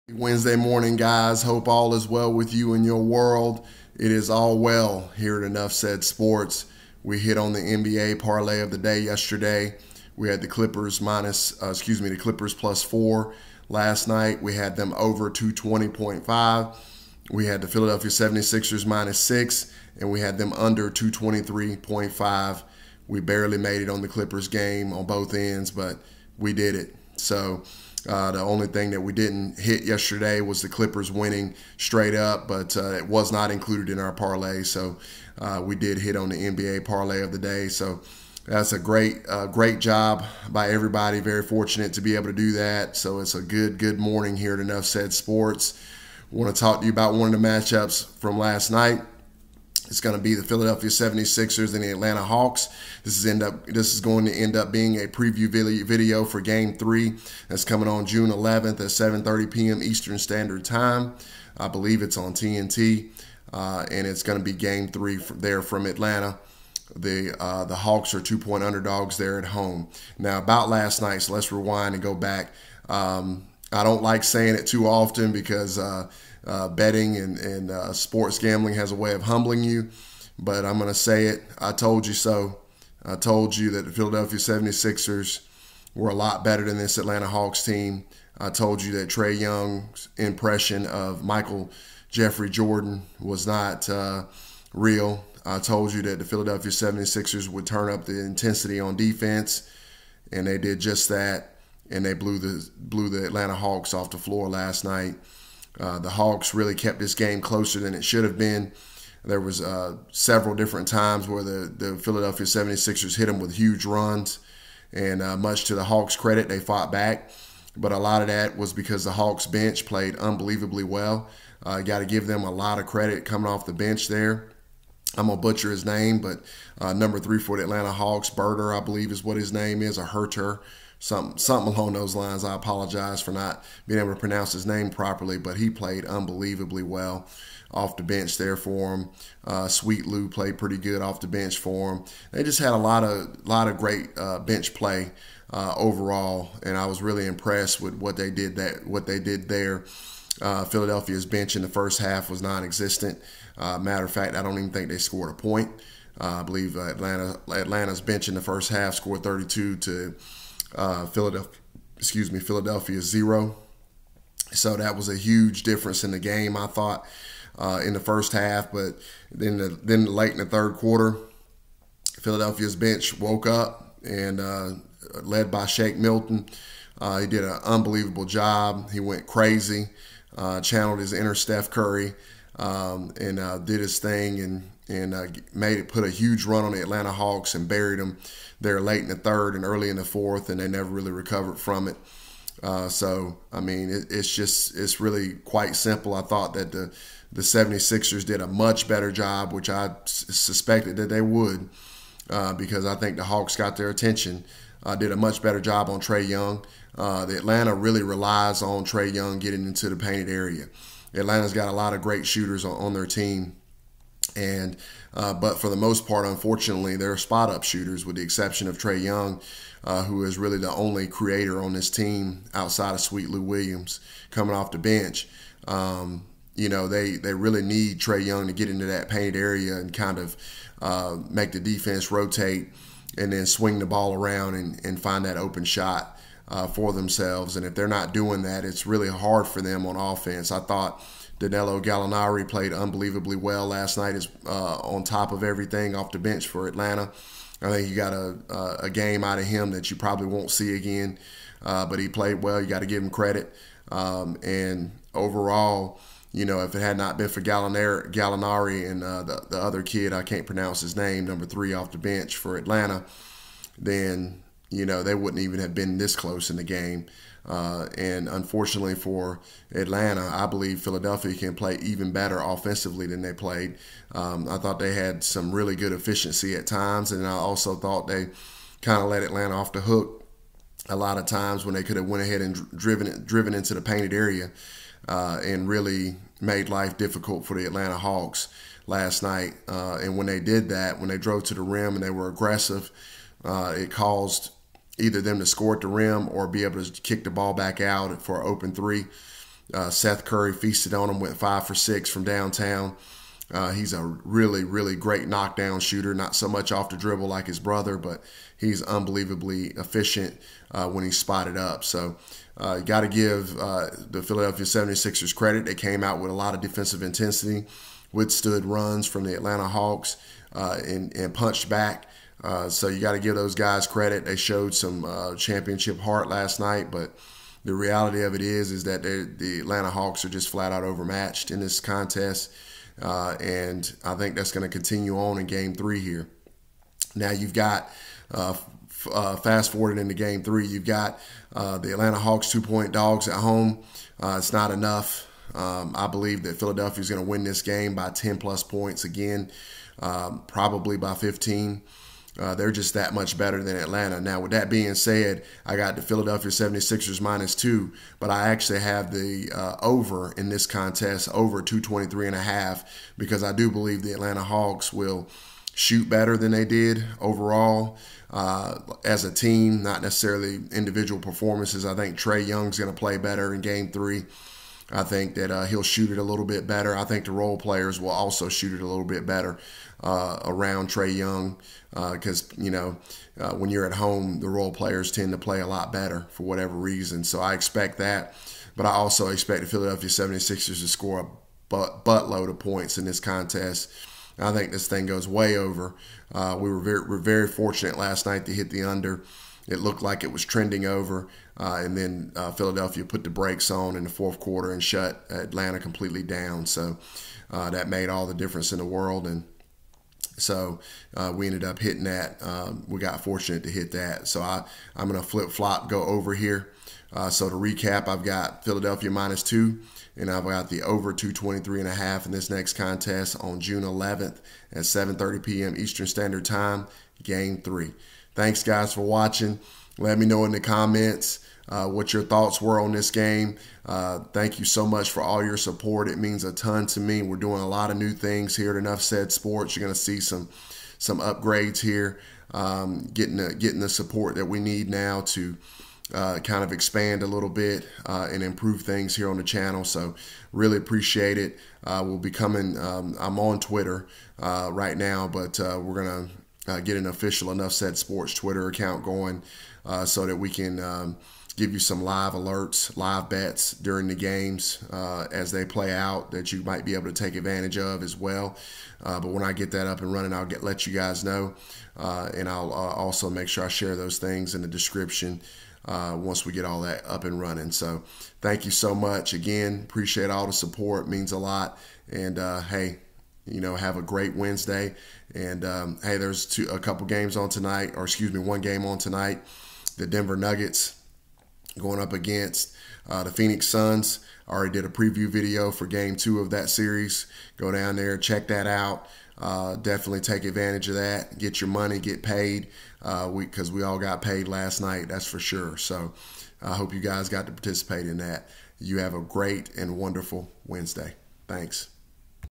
76ers vs Hawks Game 3 06/11/2021 Preview & Prediction
Sixers crush Hawks again on the road & go up 2-1 in the series. Pick 76ers-2, & over 223.5 total points